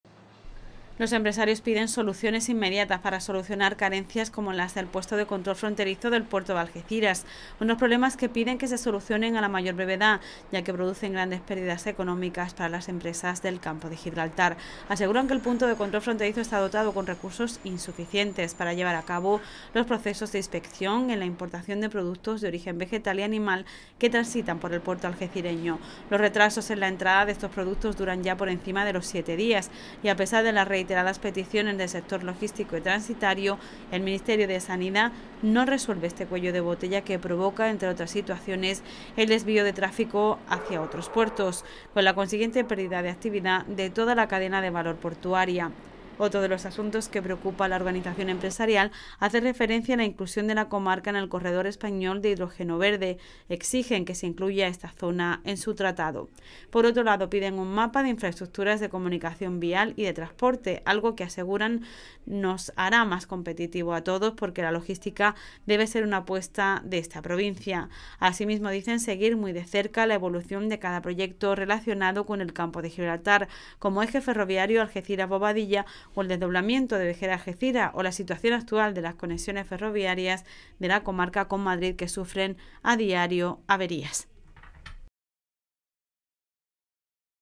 La CEC reclama un gran consenso para aumentar la competitividad del Campo de Gibraltar
La Confederación de Empresas de la provincia de Cádiz ha aprobado reclamar a las instituciones públicas a resolver los graves problemas que afectan a la competitividad empresarial del Campo de Gibraltar . Consideran que no se pueden admitir más agravios con este territorio, que ya contribuye notablemente en la generación de riqueza y empleo.